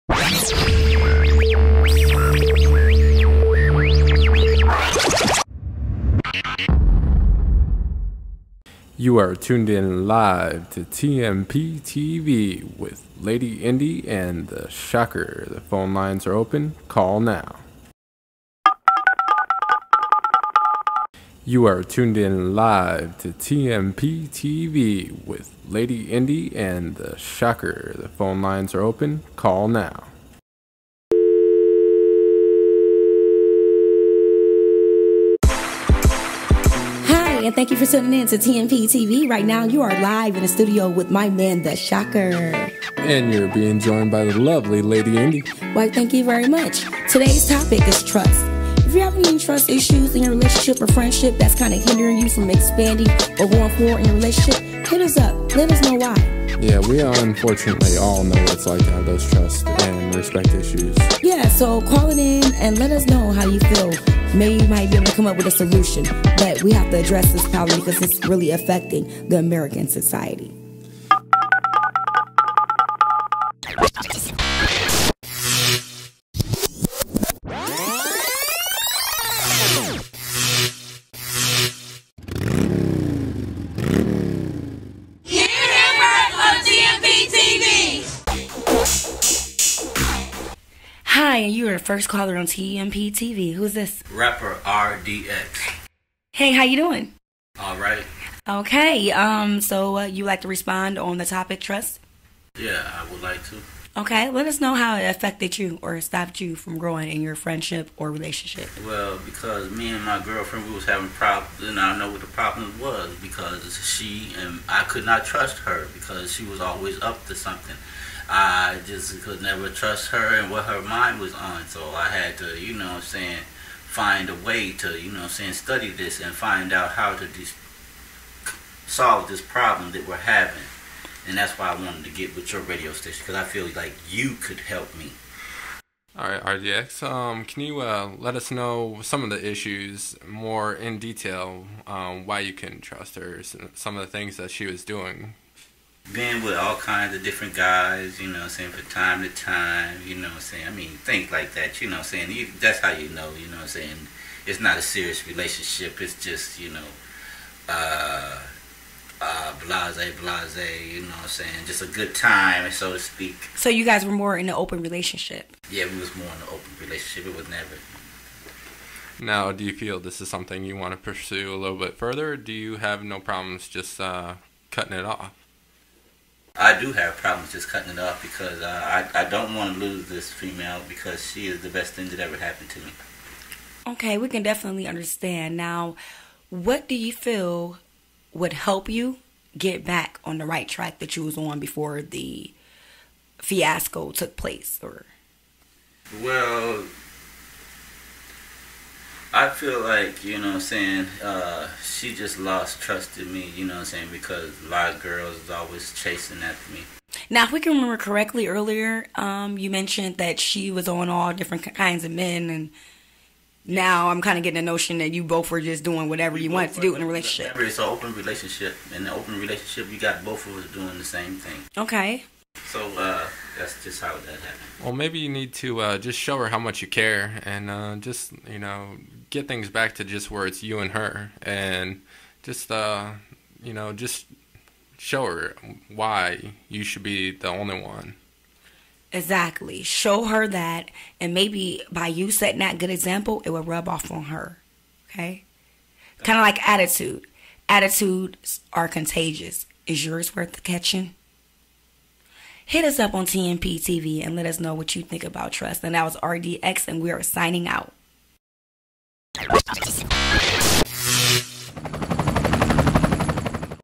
TMPTV TRUST THE ANSWER EPISODE 2
RAPPER RDX RESPONDS TO THE TOPIC TRUST LIVE WITH LADY INDY AND THE SHOCKER TMPTV  CALL US @ 260 33 TMPTV  WE ARE STILL TAKING CALLS ON THIS TOPIC OR JUST HIT US UP TO LEAVE A SHOUT OUT .  .